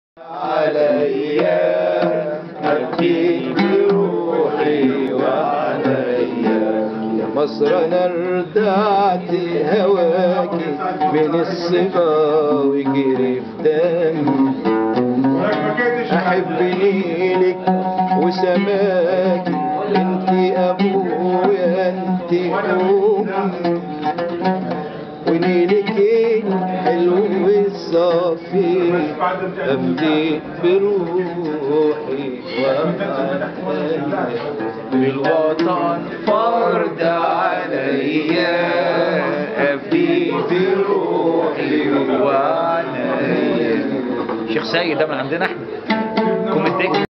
Oud in Little Egypt
Impromptu oud playing in El Karnak in Astoria, Queens (aka Little Egypt) the eve before FORMER President Mubarak announced his resignation.